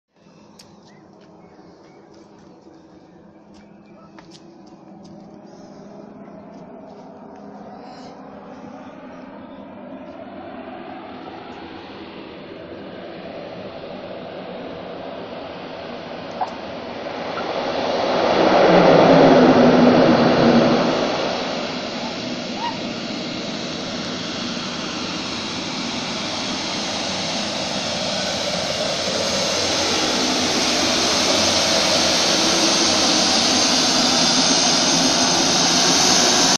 A plane lands at Lukla airport, Lukla, Nepal.
Tenzing–Hillary Airport  also known as Lukla Airport (alt. 2845m) is one of the most extreme airport. And a trek to Everest base camp starts for the most people with a landing at Lukla Airport.